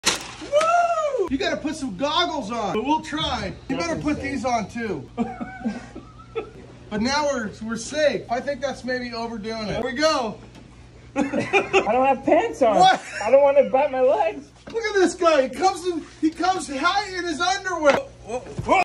Rookie Saving Snake Eggs 😂
I make it look easy pulling reticulated pythons eggs 😂 the rookies have to suit up 🥽